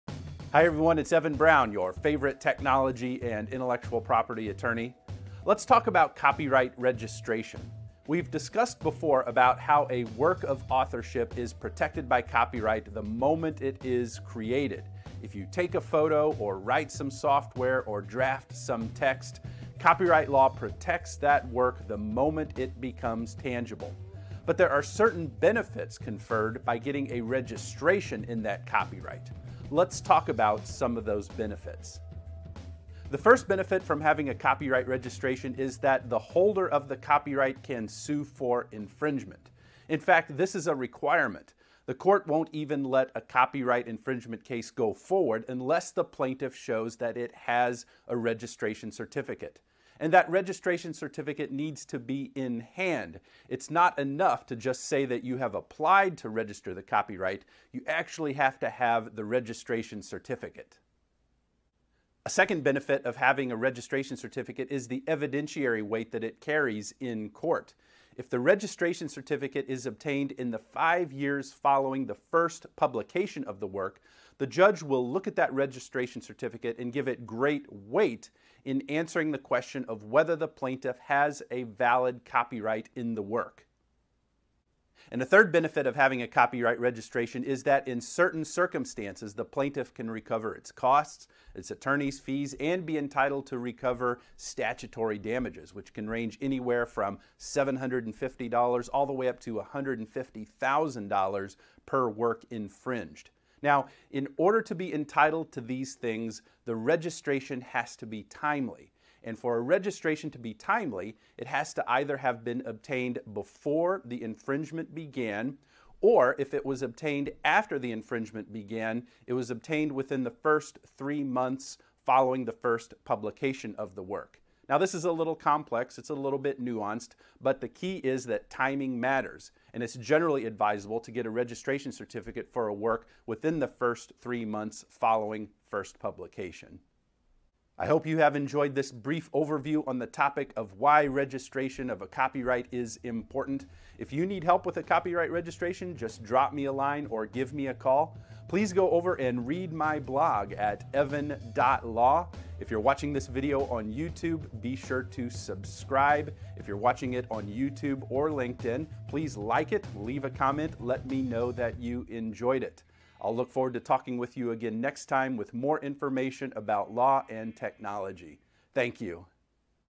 Why register your copyright? | Evan Brown
A work of authorship is protected by copyright at the moment it is created. If you take a photo or write some software or draft some text, copyright law protects that work the moment it becomes fixed in a tangible medium of expression for more than a transitory duration. You do not need a copyright registration to own a copyright. It exists automatically. But there are certain benefits conferred by getting a copyright registration.

Copyright registration allows the holder to sue for infringement.
One benefit from having a copyright registration is that the holder of the copyright can sue for infringement. And that registration certificate needs to be in hand — it is not enough for the plaintiff to have just applied to register the copyright. It actually needs to have the registration issued.

Better evidence.
A second benefit of having a copyright registration certificate is the evidentiary weight that it carries in court. If the registration certificate is obtained in the five years following the first publication of the work, the judge will look at that registration certificate and give it great importance in ascertaining whether the plaintiff has a valid copyright in the work.

Statutory damages and attorney’s fees.
A third benefit of having a copyright registration is that in certain circumstances the plaintiff can recover its costs and attorneys fees, and be entitled to recover statutory damages. These damages can range anywhere from $750 all the way up to $150,000 per work infringed.

To be entitled to these things the plaintiff must have obtained a timely copyright registration. To be timely, it has to either have been obtained before the infringement began, or if it was obtained after the infringement began, it was obtained within the first three months following the first publication of the work. The key is that timing matters — it is generally advisable to get a registration certificate for a work within the first three months following first publication.